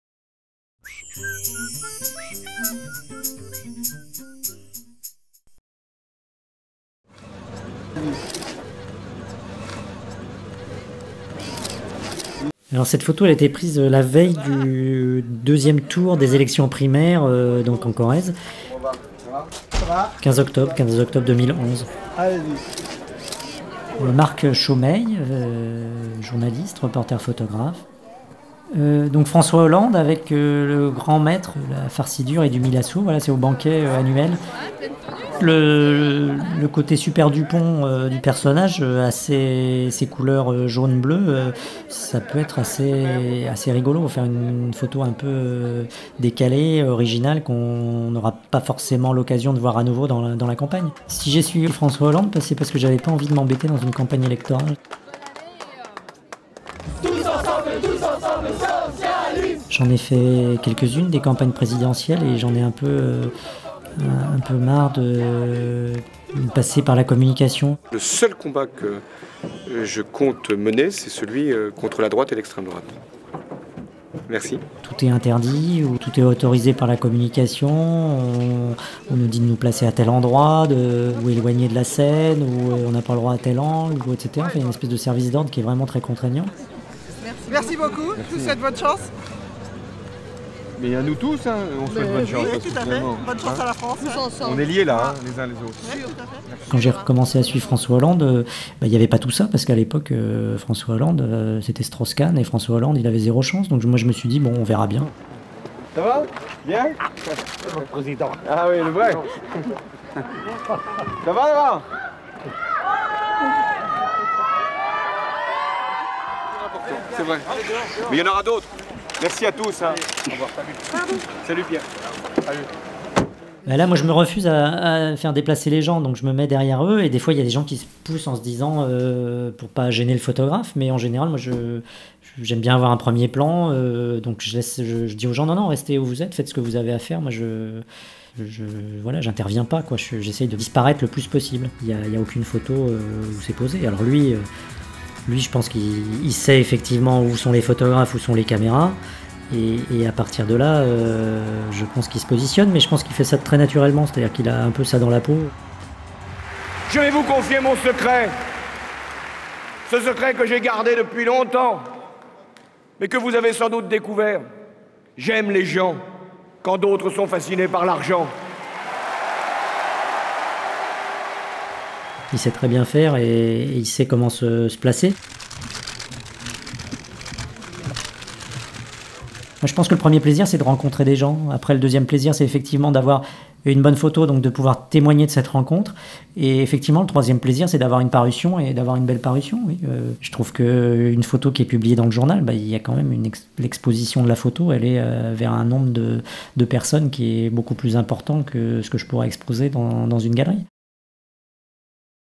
Marc Chaumeil photographie de François Hollande
Marc Chaumeil suit François Hollande depuis plusieurs années.
Toujours avec une réflexion sur l'angle qu'il veut donner à sa photo, son cadrage et la lumière, il utilise ce qui se présente pour arriver à nous faire sentir l'ambiance.
Cette photo prise en Corrèze  le 4 octobre 2011, à la veille du deuxième tour des élections primaires du parti socialiste a fait l'objet d'une publication dans le journal Marianne.